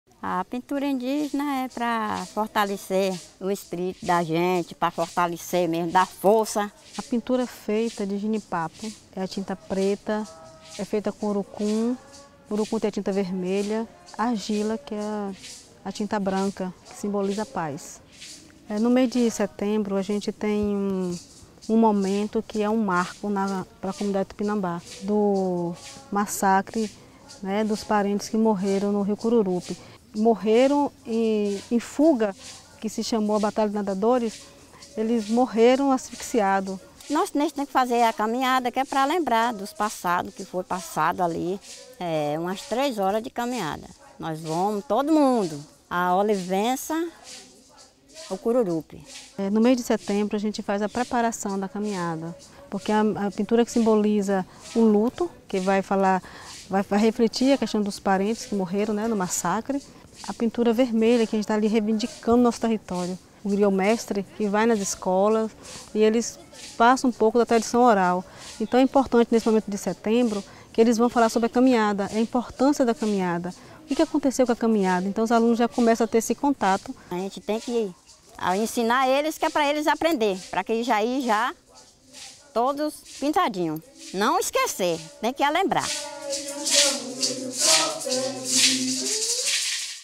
Jornada Tupinambá
Este vídeo conta um pouco da história dos Tupinambás de Olivença, na Bahia. Mostra um pouco da aldeia e seus costumes, a escola e sua continuada luta por respeito, dignidade e cidadania.